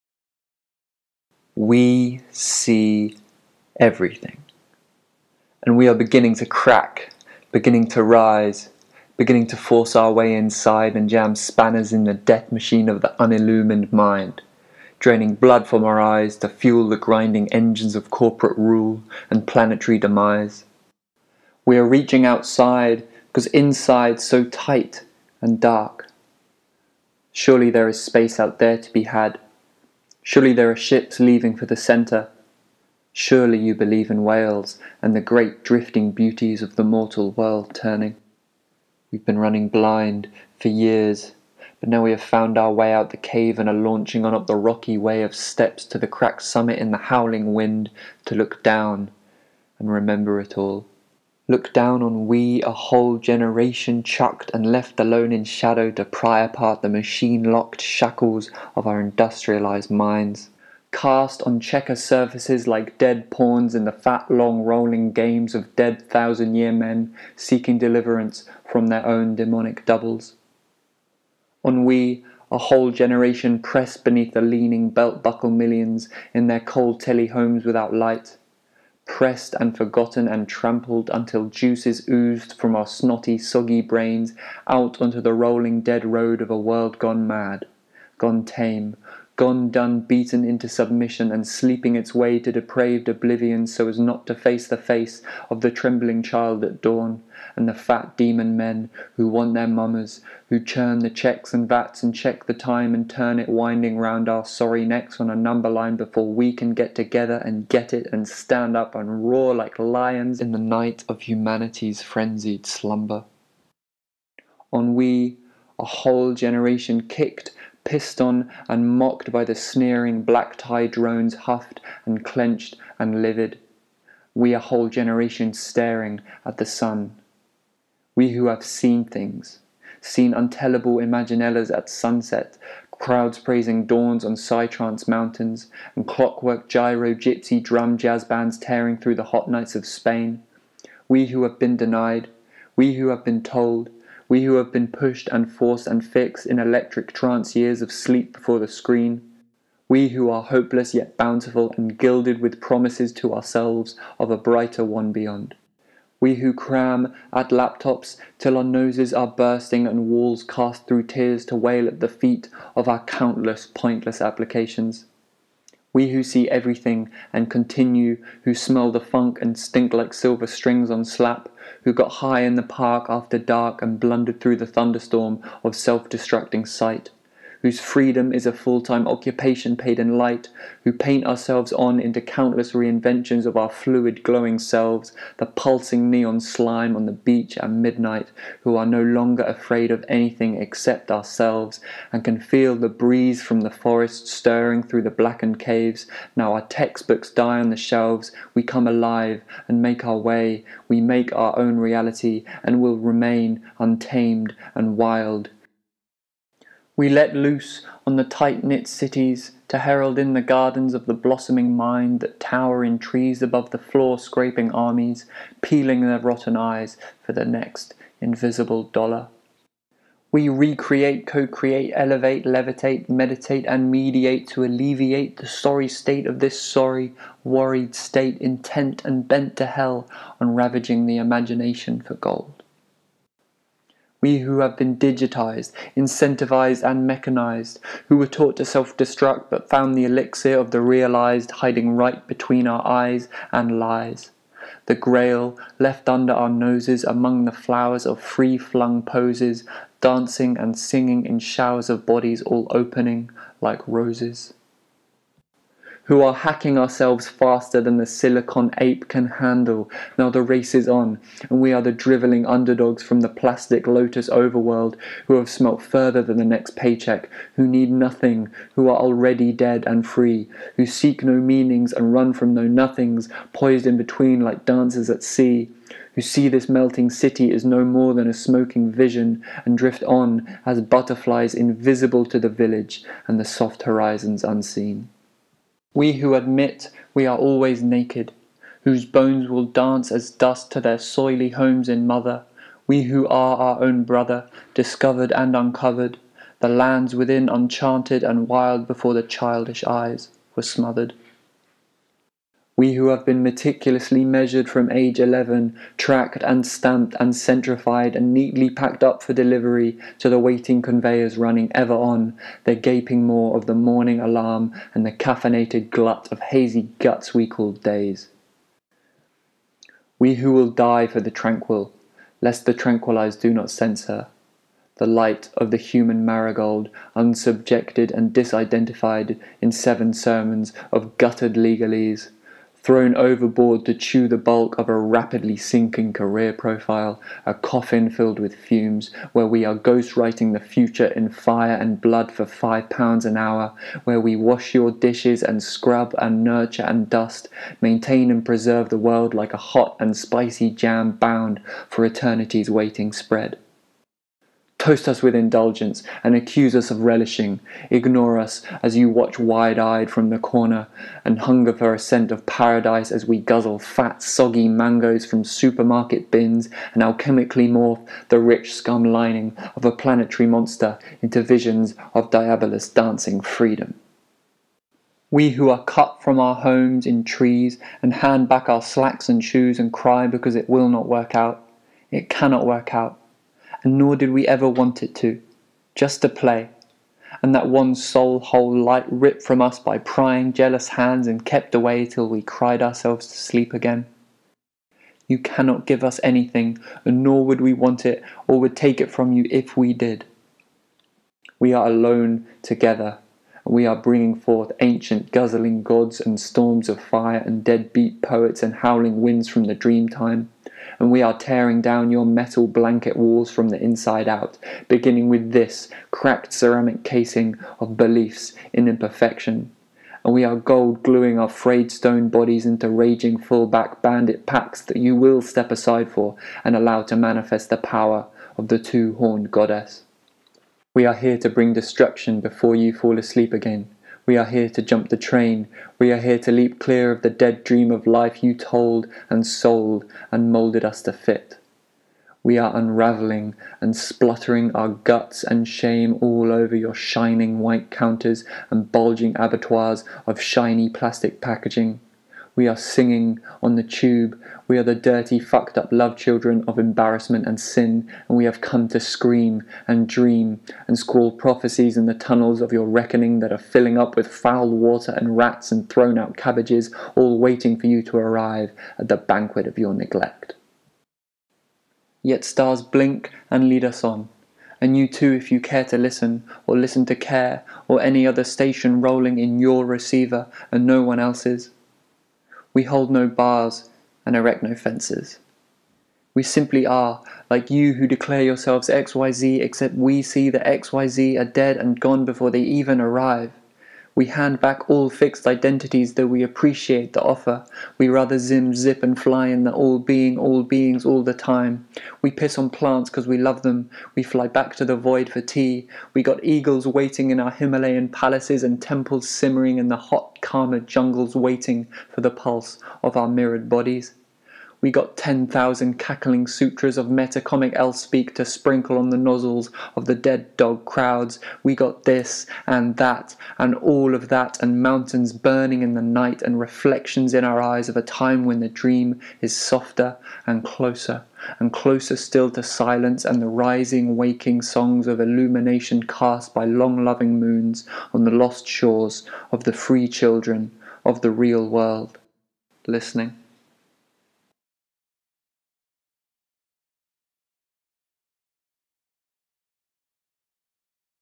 Will Franks - We See Everything
We See Everything - a spoken word poem for a generation on the rise

CC for subtitles

Original artwork